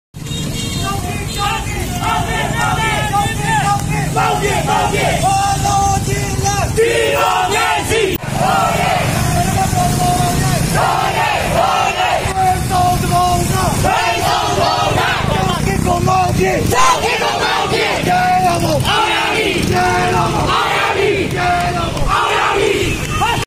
ရွှေဝါရောင်တော်လှန်ရေး ၁၅နှစ်ပြည့်အထိမ်းအမှတ် မန္တလေးတွင် ဆန္ဒပြ
၂၀၀၇ ရွှေဝါရောင်တော်လှန်ရေး ၁၅နှစ်ပြည့် အထိမ်းအမှတ်အဖြစ် အဖမ်းအဆီး၊ အစစ်အဆေး တင်းကျပ်နေသည့်ကြားမှ မန္တလေးမြို့တွင် ယနေ့(စက်တင်ဘာ ၂၅) ညဦးပိုင်းက ချီတက်ဆန္ဒပြခဲ့ကြသည်။

လောင်စာဆီဈေးနှုန်း မြင့်တက်ရာမှစ၍ သံဃာတော်များ ဦးဆောင်သည့် ရွှေဝါရောင်တော်လှန်ရေး ပေါ်ပေါက်ခဲ့သည်။ ဒီမိုကရေစီကိုထောက်ခံသော ရွှေဝါရောင်တော်လှန်ရေးကို စစ်တပ်က အကြမ်းဖက် သတ်ဖြတ်နှိမ်နင်းခဲ့သည်။

နွေဦးတော်လှန်ရေး ခေါ်  ၂၀၂၁ စစ်အာဏာသိမ်းမှုကို ဆန့်ကျင်သည့် ပြည်သူလူထုကိုလည်း စစ်တပ်က ထိုနည်းလည်းကောင်း ကျူးလွန်နေသည်။